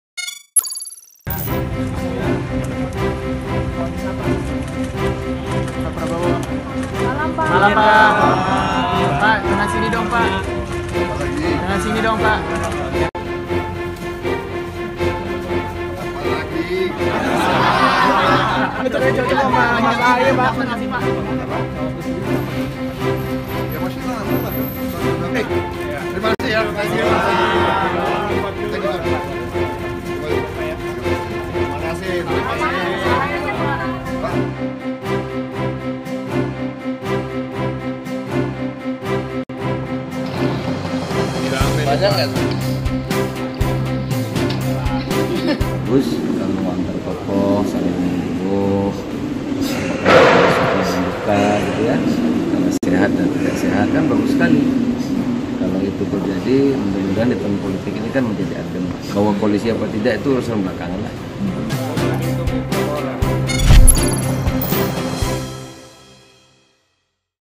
Pertemuan Prabowo-SBY, Pan: Semoga Politik Makin Adem
Ketum Gerindra Prabowo Subianto menjenguk Ketum Partai Demokrat Susilo Bambang Yudhoyono di Rumah Sakit Pusat Angkatan Darat (RSPAD) Gatot Subroto, Jakarta. Ketum PAN Zulkifli Hasan menyambut baik pertemuan itu yang bisa meredakan isu politik.